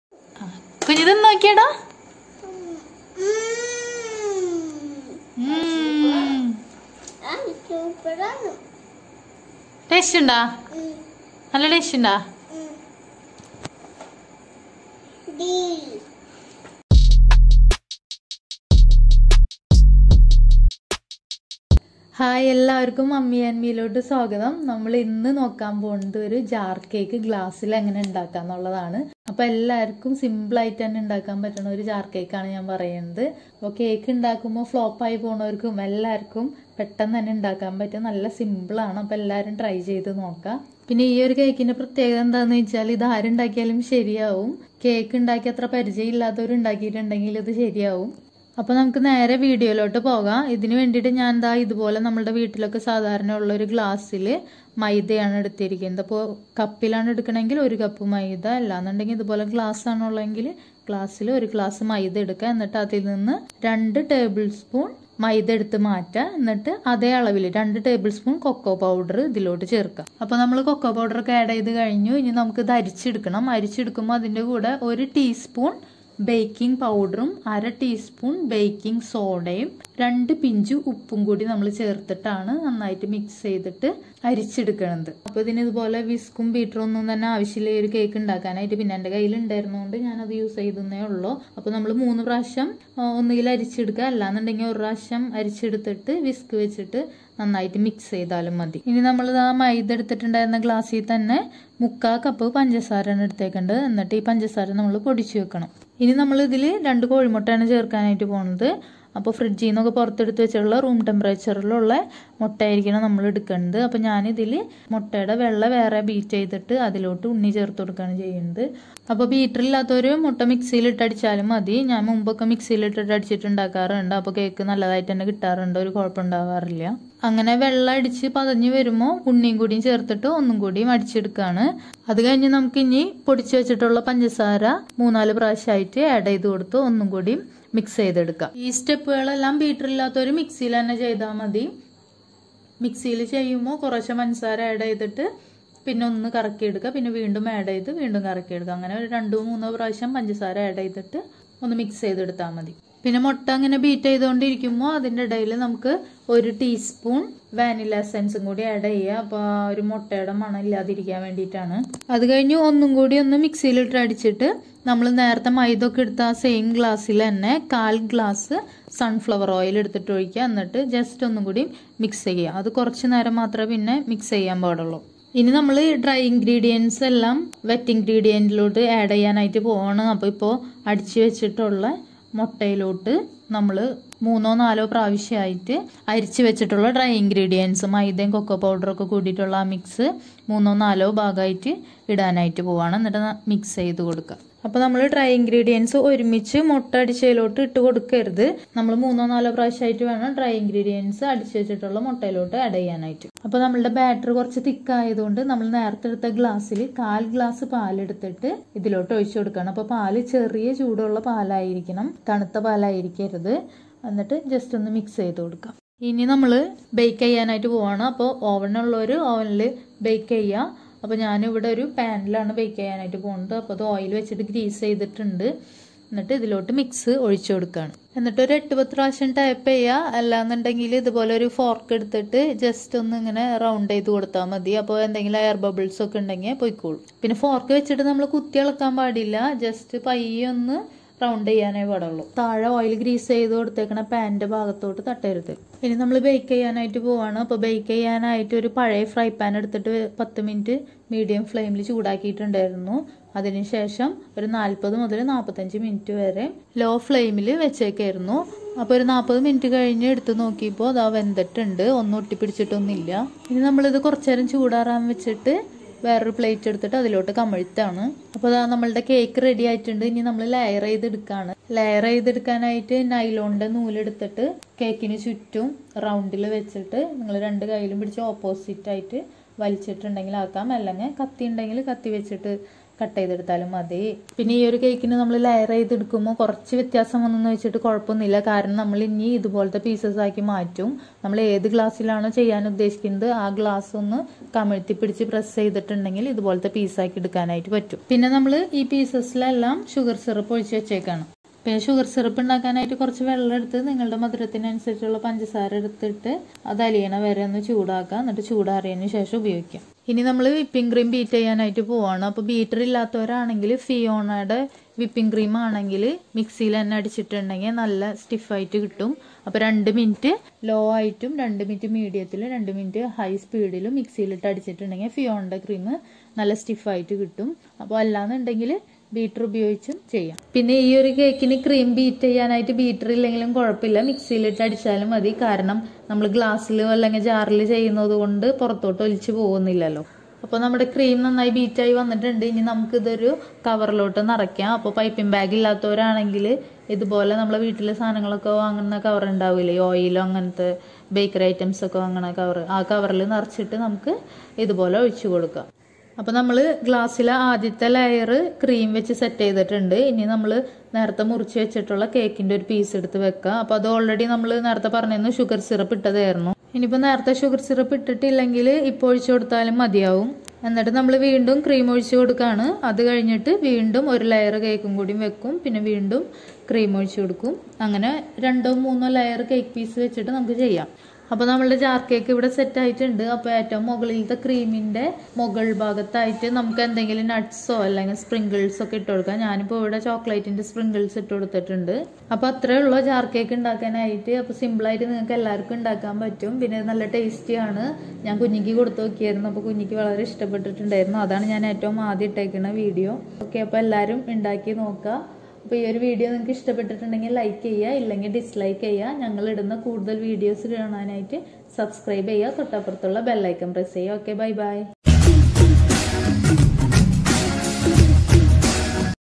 Jar Cake |Without Oven |Simple Recipe| Mummy & Me Ayrapiii |Vlog 7
This video tutorial is about a simple Jar Cake preparation.Anybody can prepare it with this recipe very simply..